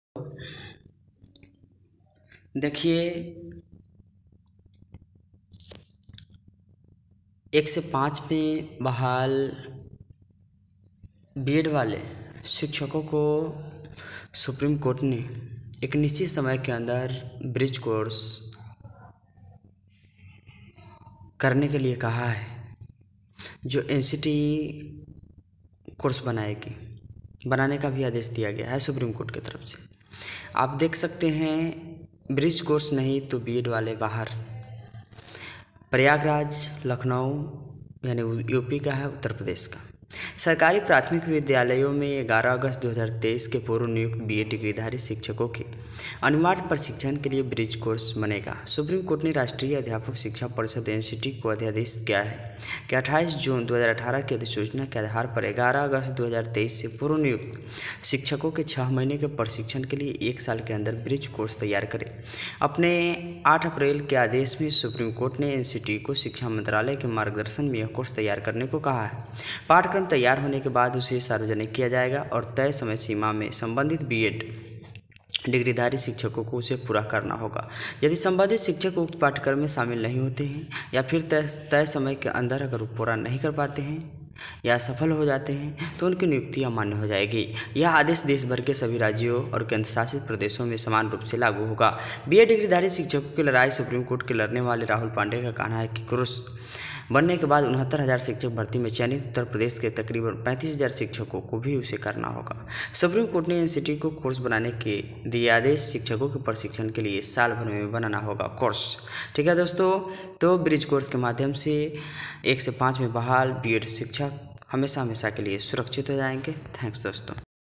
सुप्रीम कोर्ट:ब्रिज कोर्स नहीं तो B.Ed. वाले कक्षा 1-5 से होंगे बाहर??NCTE को कोर्स बनाने का आदेश??
ncte suprimcourt b.ed 1to5 bridgecourse highcourt patna bihar up सुप्रीम कोर्ट:ब्रिज कोर्स नहीं तो B.Ed. वाले कक्षा 1-5 से होंगे बाहर??NCTE को कोर्स बनाने का आदेश??